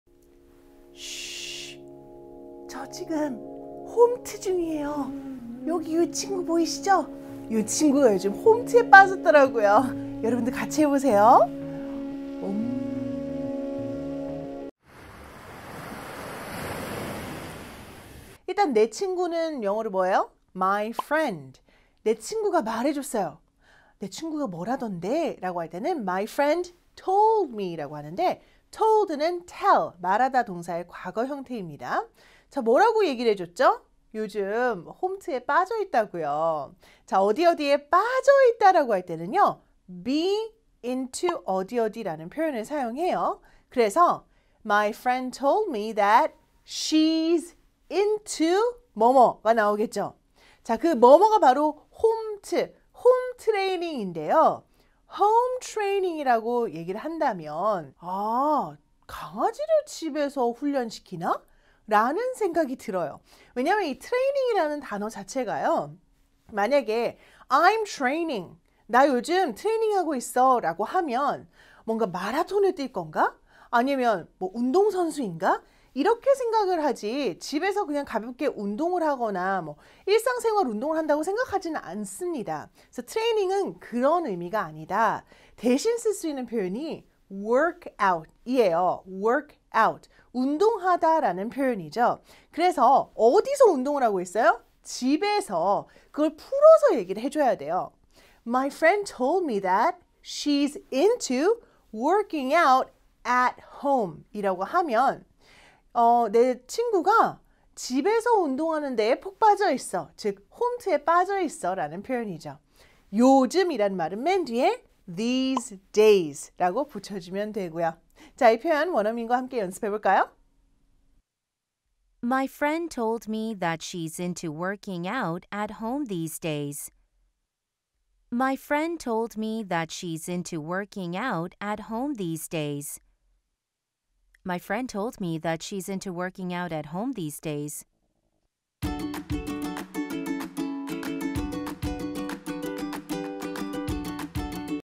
'홈트'는 영어로?🚴‍♀️🤸‍♀️ (영어 표현, 초급 영어, 영어회화, 영어 스피킹)
📕 오늘의 문장
친구가 요즘 홈트에 빠졌다고 하더라고.
My friend told me that she's into working out at home these days.

📕 단어 및 표현
tell 말하다
be into ~에 빠지다
work out 운동하다
these days 요즘

💡 TIP
요즘 코로나로 인해서 홈트를 하시는 분들이 많은데요. ‘홈트’는 영어로 working out at home 이라고 표현할 수 있습니다. 그렇다면 ‘홈트에 빠지다’는 영어로 어떻게 표현할 수 있는지 오늘 영상을 통해 배워볼까요?
⚡꾸준한 영어 공부 습관 기르기

⚡매일 아침 119 영상을 메일로 받아보고 싶다면?